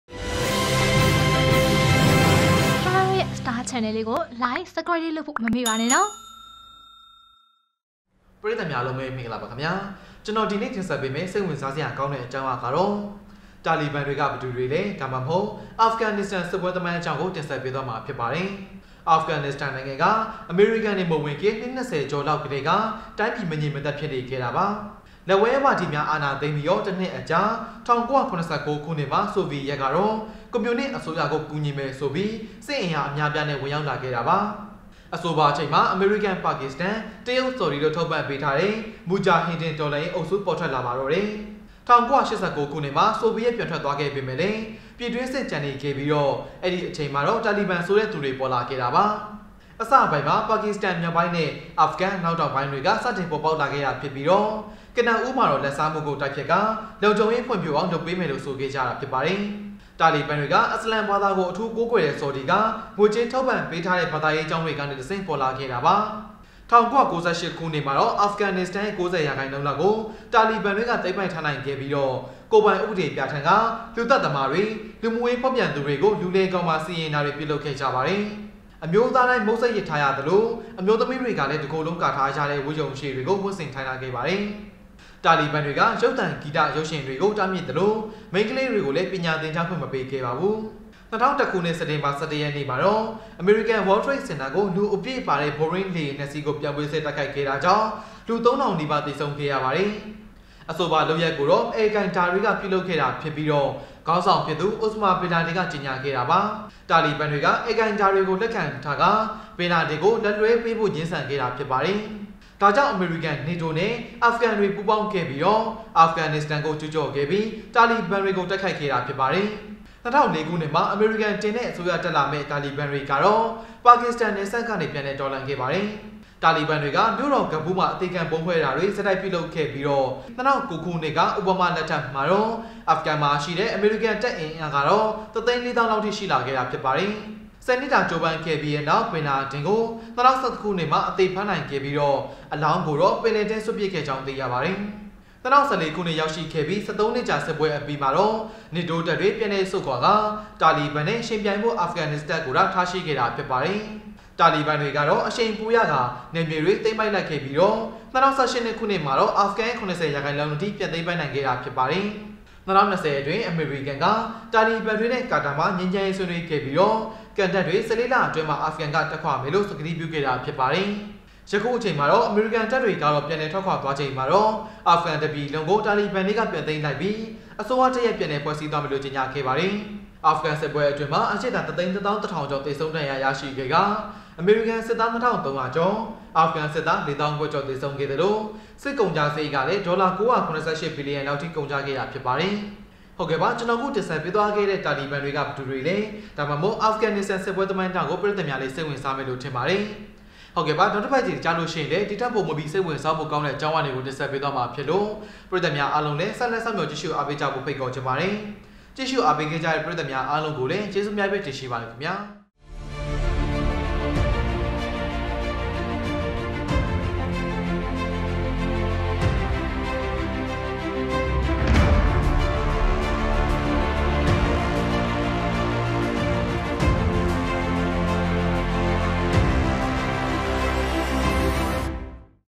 တာလီဘန်တွေက ဘယ်သူတွေလဲ(သို့မဟုတ်)အာဖဂန်နစ္စတန် စစ်ပွဲသမိုင်းကြောင်း - Star Channel TV
အာဖဂန်နစ္စတန်နိုင်ငံဟာ အမေရိကန်တွေ မဝင်ခင် နှစ်၂၀ကျော်လောက်ကတည်းက တိုင်းပြည် မငြိမ်မသက် ဖြစ်နေခဲ့တာပါ။

လက်ဝဲဝါဒီများ အာဏာသိမ်းပြီး ၁နှစ်အကြာ ၁၉၇၉ခုနှစ်မှာ ဆိုဗီယက်က ကွန်မြူနစ်အစိုးရကို ကူညီမယ်ဆိုပြီး စစ်အင်အားအများအပြားနဲ့ ဝင်လာခဲ့ပါတယ်။